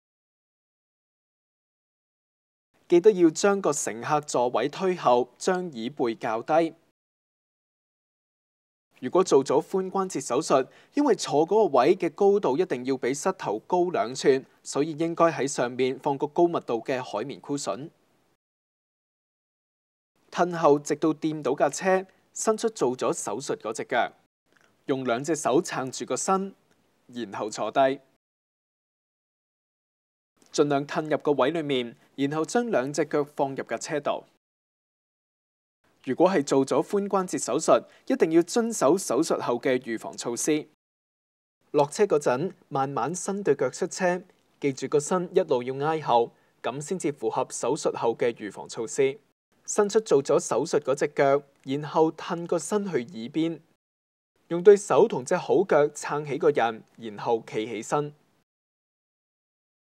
Cantonese - 上車、下車 (Getting In and Out of a Car)
Demonstrates how to get in and out of car after hip surgery.